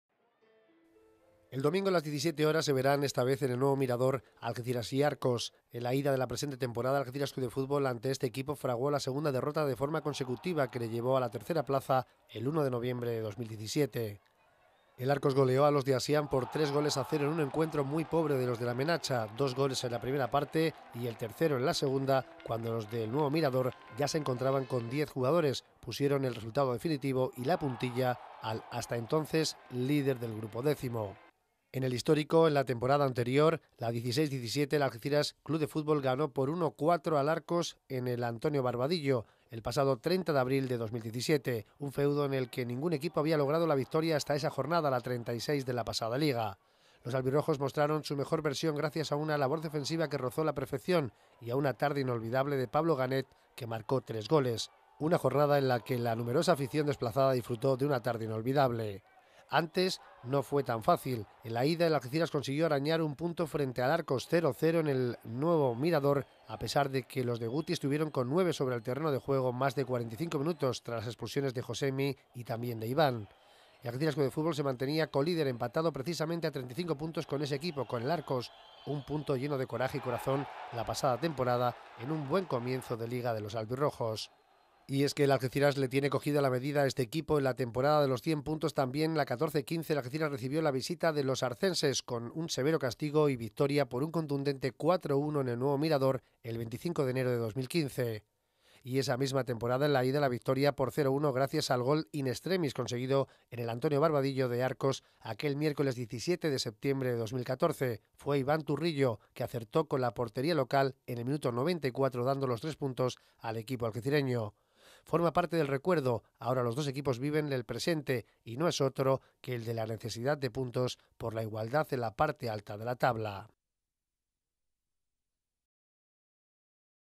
Algeciras – Arcos, encuentros con mucha historia
Algeciras CF y Arcos CF llevan varias temporadas encontrándose en Tercera División y siempre con una rivalidad deportiva que crea interés cada vez que se enfrentan. Los errores de los arcenses en la segunda vuelta los separan 10 puntos de los puestos de liguilla, por lo que aunque matematicamente es posible, lo tienen muy difícil en una temporada en la que se creó un equipo para ascender y están en el puesto 10 de la tabla.